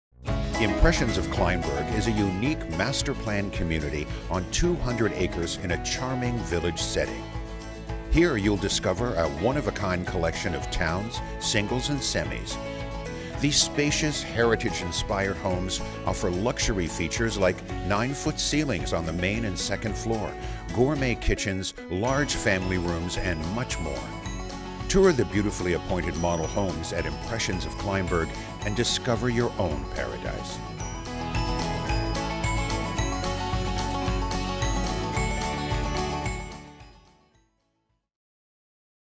Paradise Homes - Impressions of Kleinburg - Toronto Video Production
Unikron proudly presents Paradise Homes - Impressions of Kleinburg

Paradise Homes has spent over 40 years building homes and communities and have remained committed to their core values. With over 13,000 homes, each home is filled with timeless design, thoughtful planning and unparalleled craftsmanship which fills them with positivity.

Video Production - Unikron, Toronto